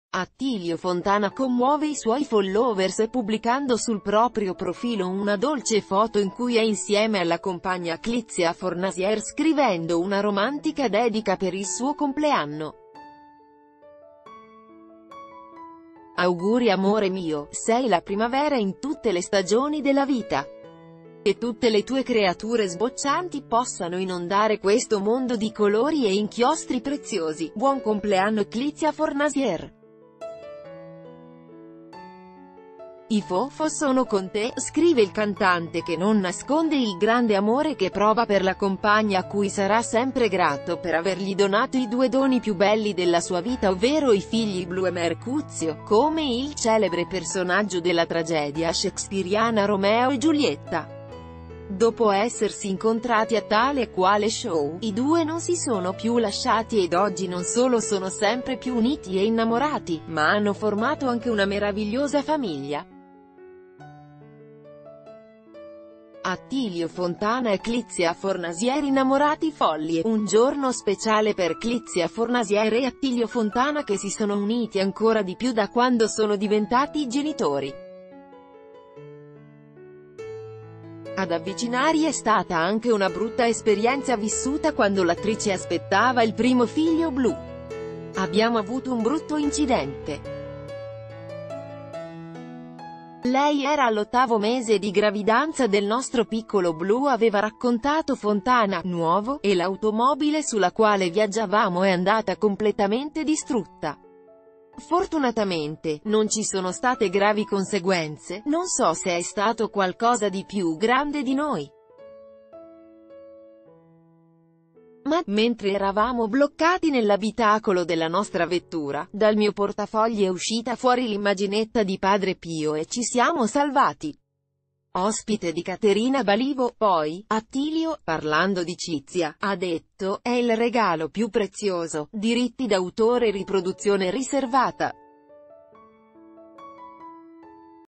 Attilio Fontana a Clizia Fornasier/ Foto, dedica d'amore: "auguri amore mio"
Attilio Fontana commuove i suoi followers pubblicando sul proprio profilo una dolce foto in cui è insieme alla compagna Clizia Fornasier scrivendo una romantica dedica per il suo compleanno. “Auguri Amore Mio, sei la primavera in tutte le stagioni della vita. Che tutte le tue creature sboccianti pos...